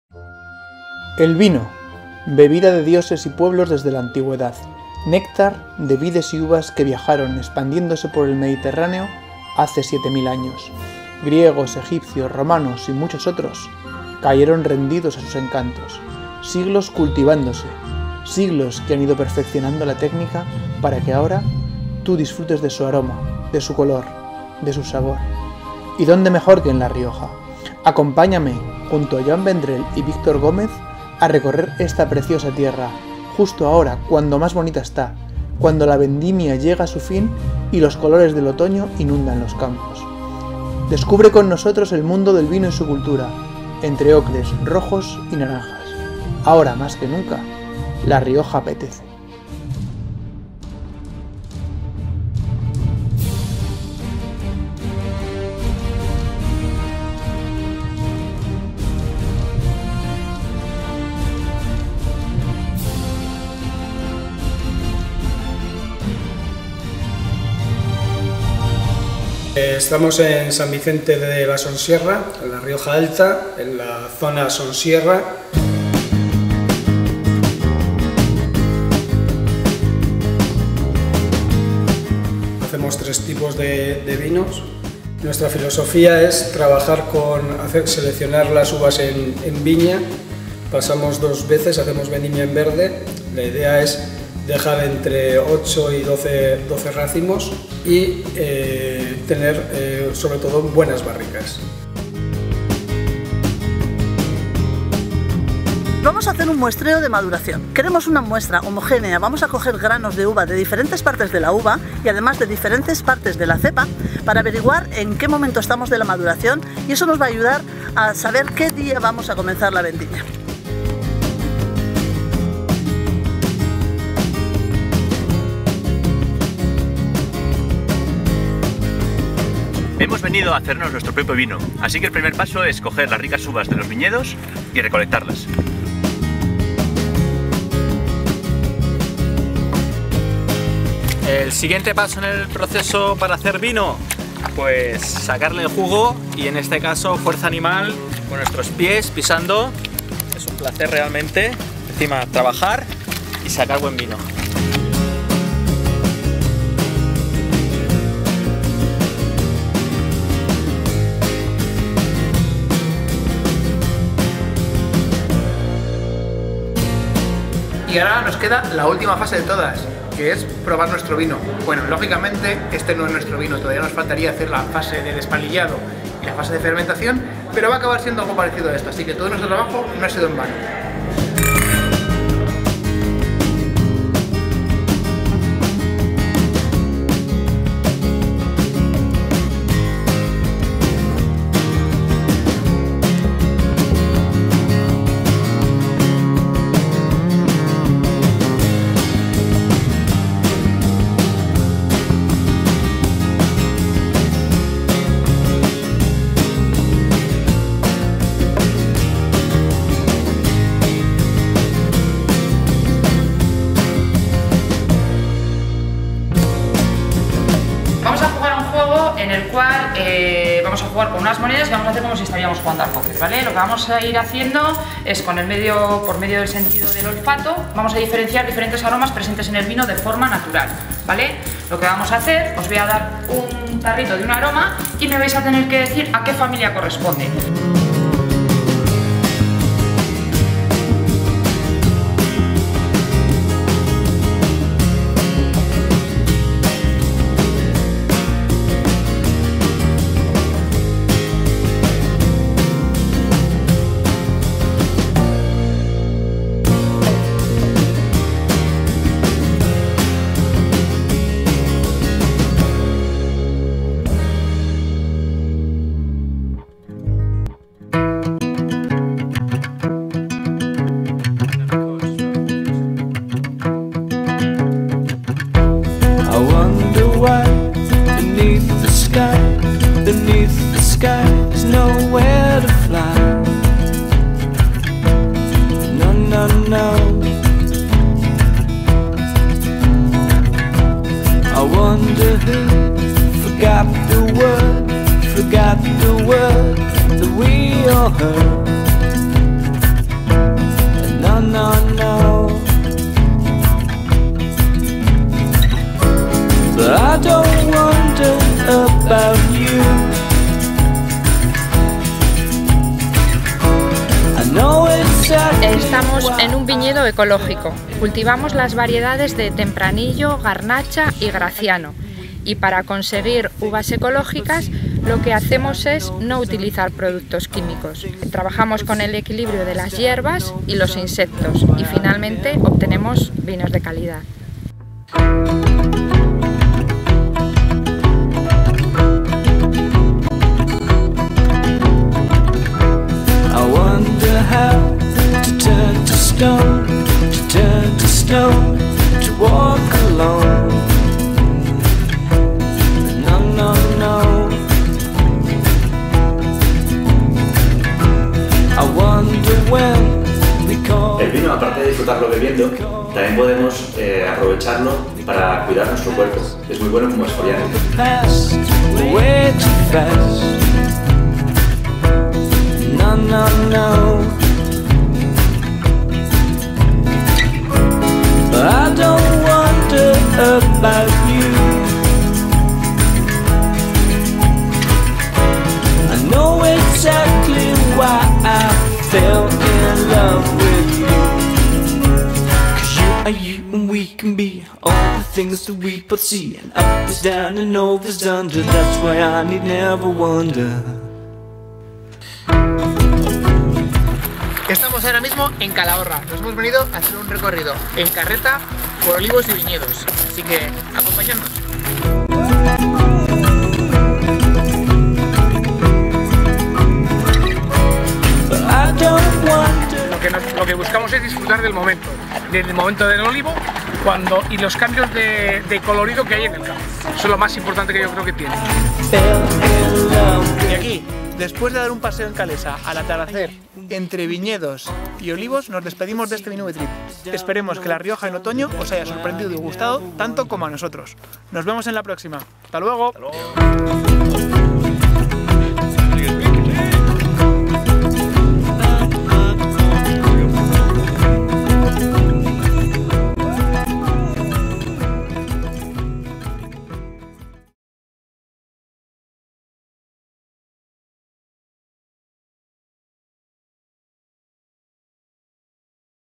Enoturismo de otoño en La Rioja-minube
La Rioja en otoño es perfecta para disfrutar de los colores de sus viñedos, y de los sabores de sus vinos. Acompaña a Ignacio Izquierdo, Joan Vendrell y Victor Gómez en su visita a estas tierras riojanas y descubre sabores y paisajes nuevos.